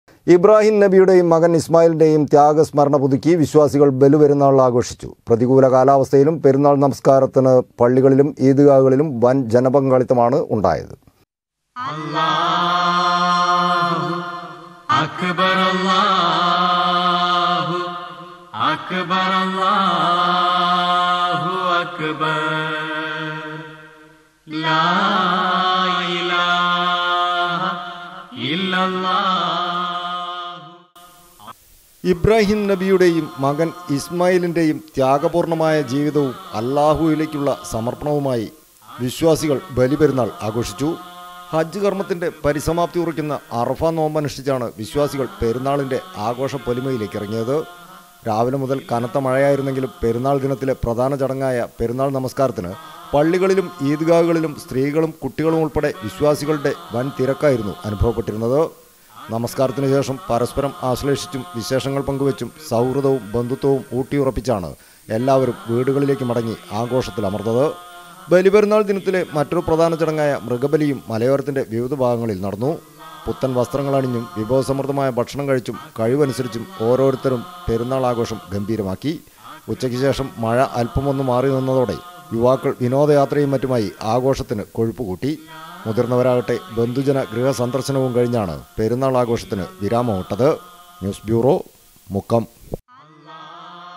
ഇബ്രാഹീം നബിയുടേയും മകൻ ഇസ്മായിലിന്റെയും ത്യാഗസ്മരണ പുതുക്കി വിശ്വാസികൾ ബലി പെരുന്നാൾ
ഇബ്രാഹീം നബിയുടേയും മകൻ ഇസ്മായിലിന്റെയും  ത്യാഗസ്മരണ പുതുക്കി വിശ്വാസികൾ  ബലി പെരുന്നാൾ ആഘോഷിച്ചു